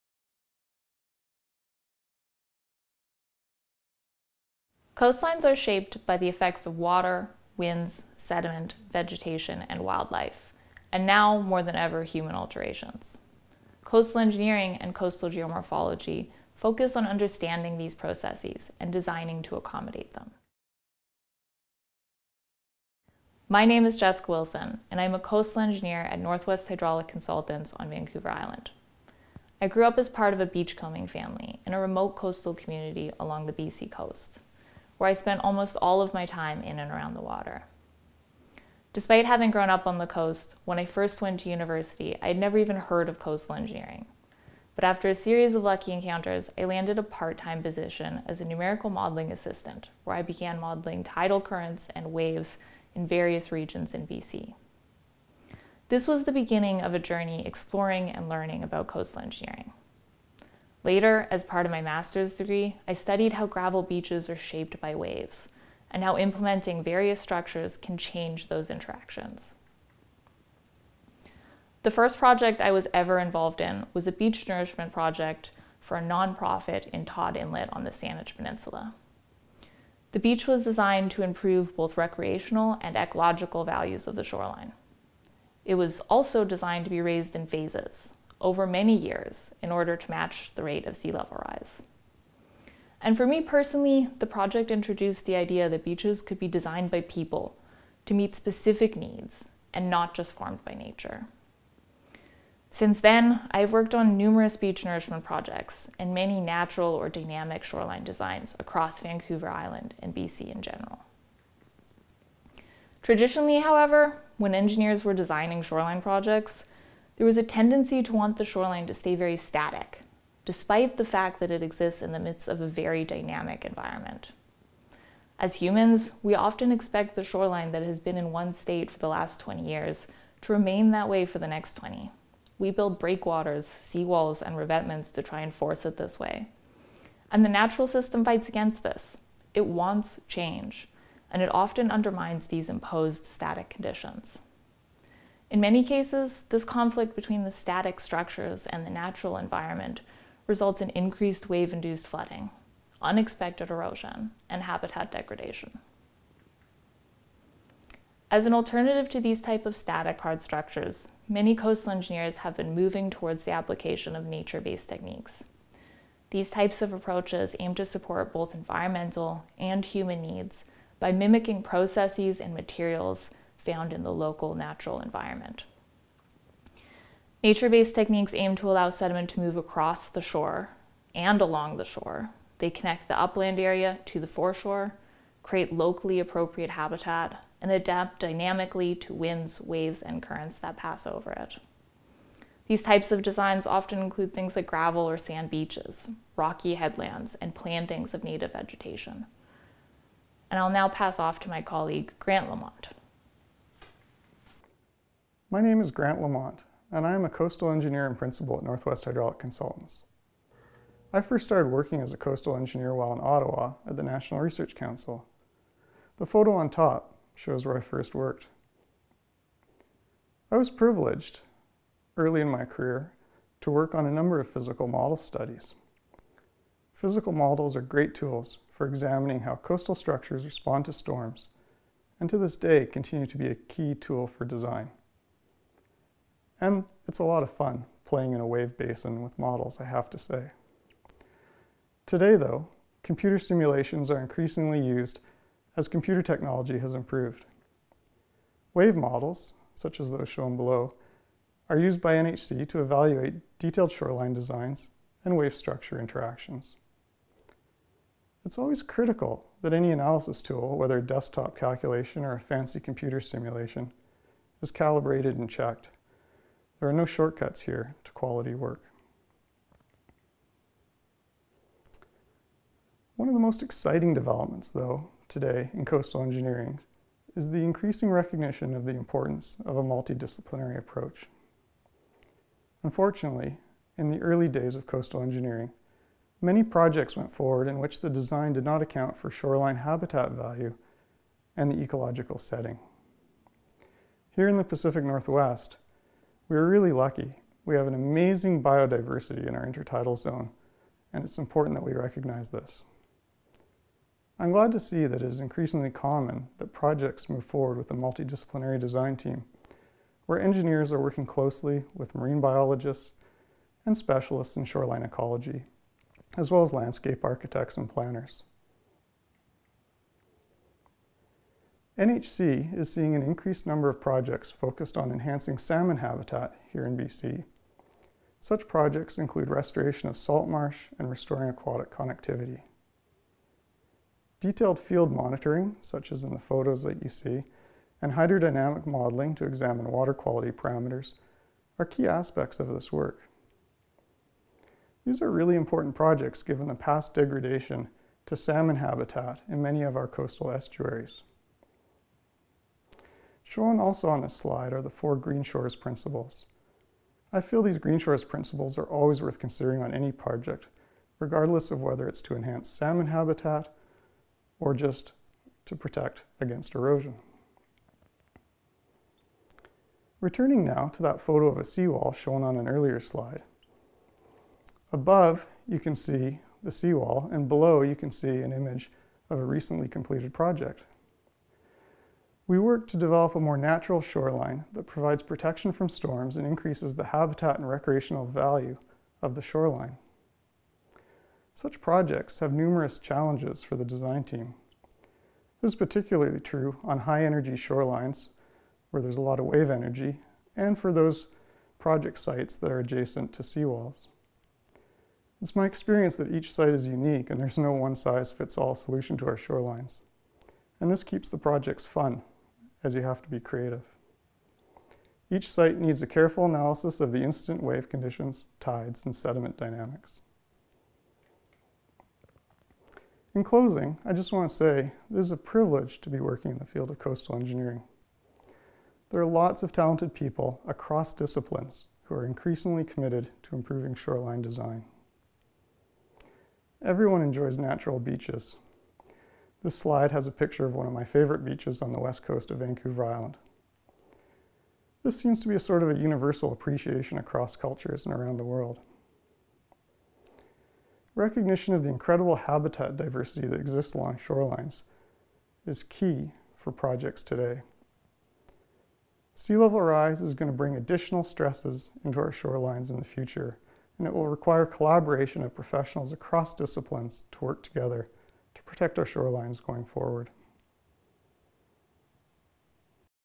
The Role of Coastal Engineering and Coastal Geomorphology in Designing Resilient Coastlines
Coastlines are shaped by the combined effects of water, winds, sediment, vegetation, and wildlife, and now – more than ever - human alterations. Coastal engineering and coastal geomorphology focus on understanding these processes and designing to accommodate them. Traditionally when engineers were designing shoreline projects, there was a tendency to want the shoreline to stay very static, despite the fact that it exists in the midst of a very dynamic environment. In the Pacific Northwest, there are many shorelines upon which seawalls and rock revetements were built that have caused significant shoreline degradation.

The implementation of successful nature-based projects such as Green Shores® for Homes and Green Shores for Shoreline Development Green Shores Home - Stewardship Centre for BC (stewardshipcentrebc.ca) requires careful consideration of all the forces that shape the shoreline. It is also important for project designs to protect the upland, riparian and foreshore while meeting the needs of the landowner for values such as aesthetics and structure safety. Today coastal engineering works as part of a multi-disciplinary team of biologists, landscape architects and other environmental specialists to consider the local conditions at the site, and how best to provide adaptation to dynamic factors such as climate change.

In this video presentation you will meet two practicing Coastal Engineers who will share their insight of how they became interested in this profession, and how they enjoy applying their skill sets and knowledge to the increasingly complex world of shoreline protection.